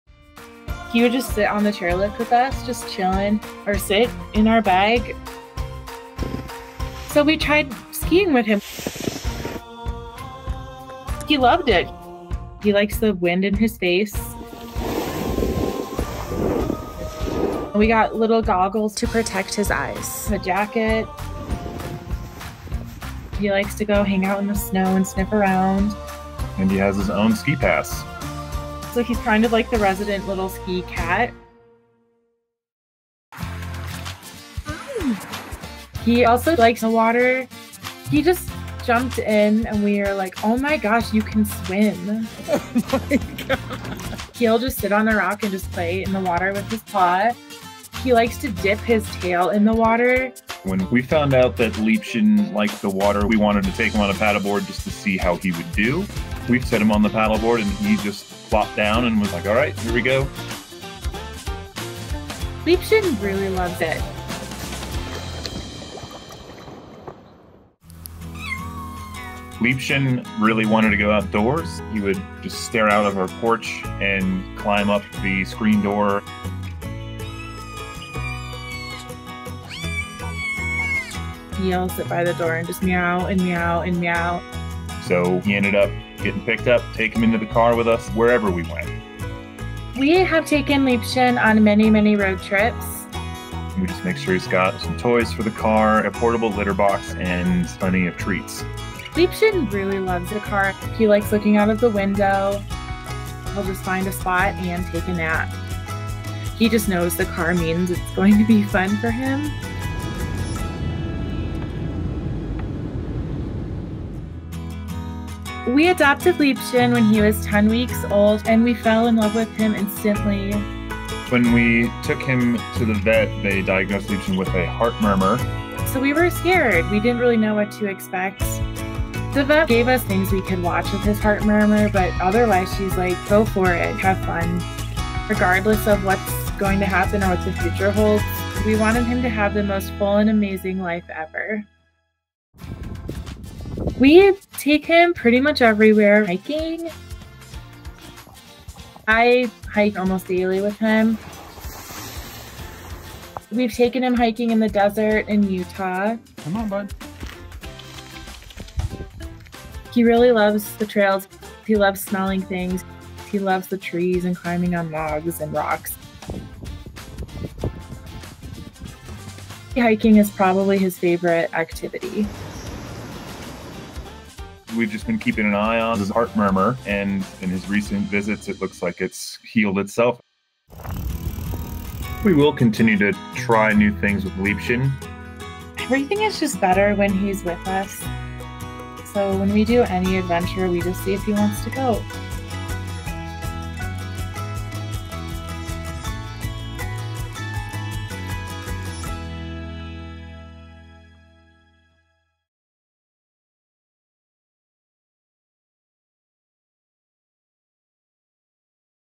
Gato ama tanto esquiar que tiene su propio boleto para la montaña | Pequeño y Valiente | El Dodo
Este gato tiene su propio pase de esquí y se sube a la puerta mosquitera de sus padres cuando está atrapado dentro 😸

¡Un agradecimiento muy especial a Liebchen, Erin y Dan! Síguelos en TikTok y Instagram